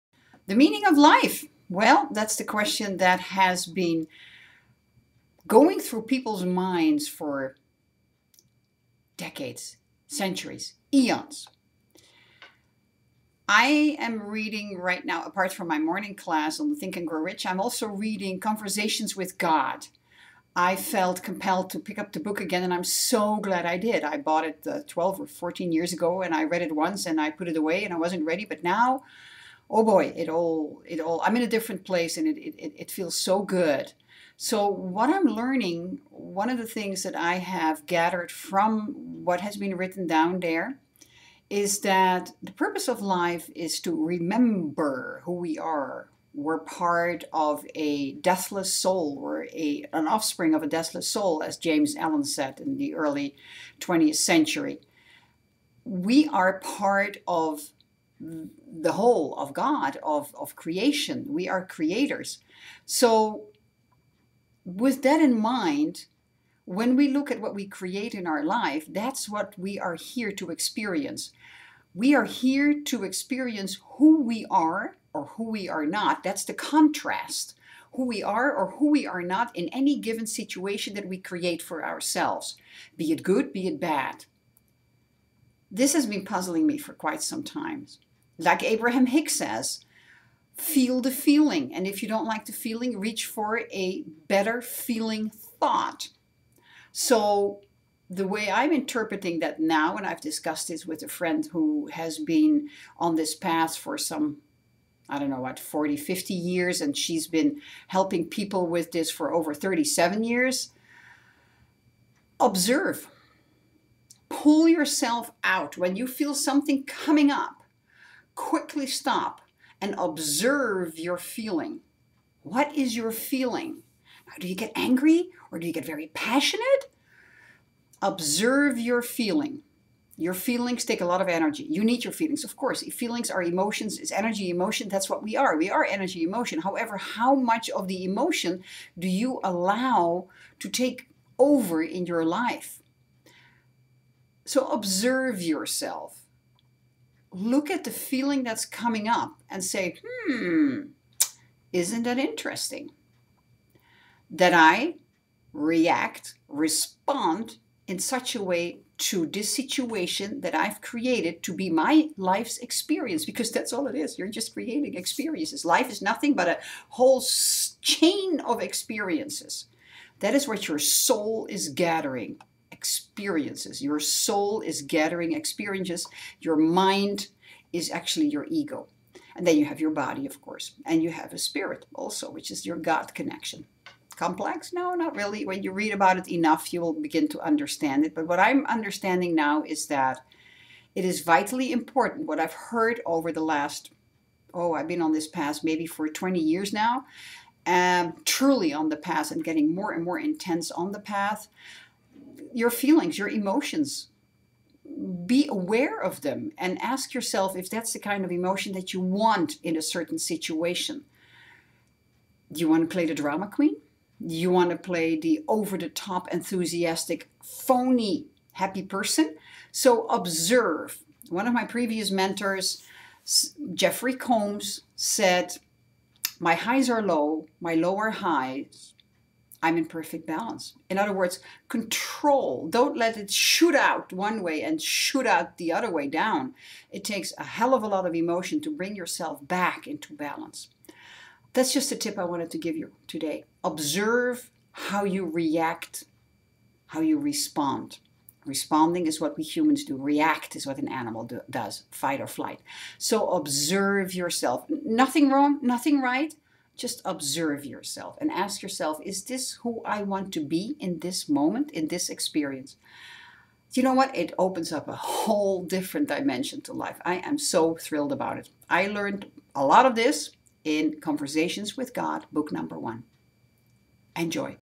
Observe yourself
Observe how you react or respond to any given situation you create and say: Hmmm isn't that interesting and ask if that is Who You Want To Be in that situation.